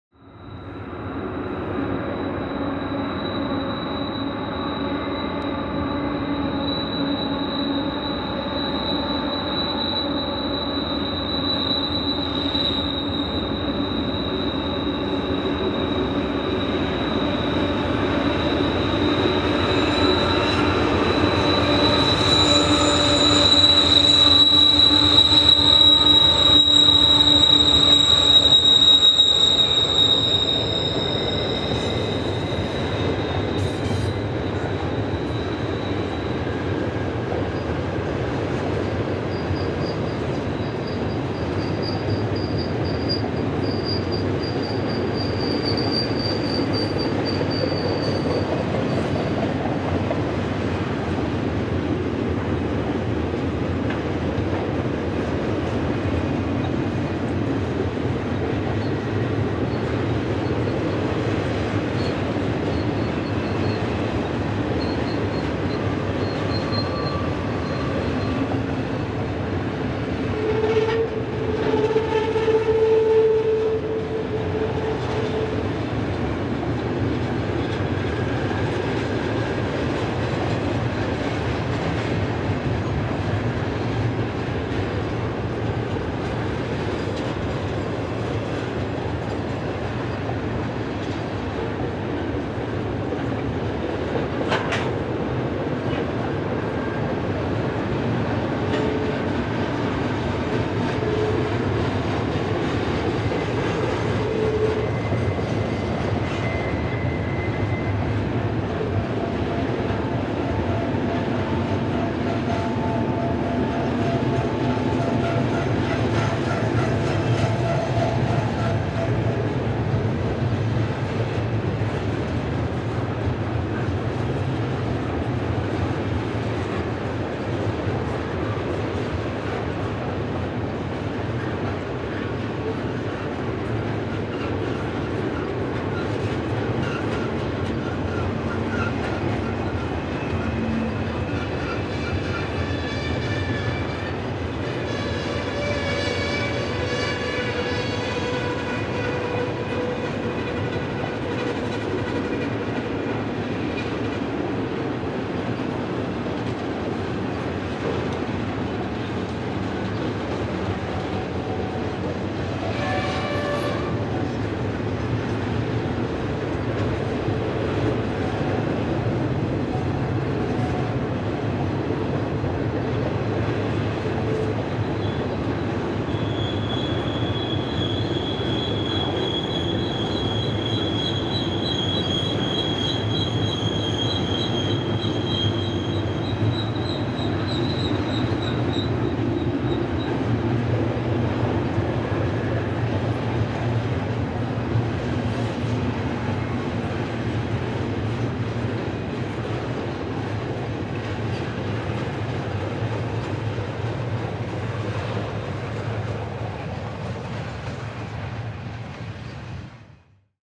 MRL/BNSF Eastbound stack crosses Austin Creek Trestle
A BNSF eastbound crosses Austin Creek Trestle as it descends Mullan Pass in March 2014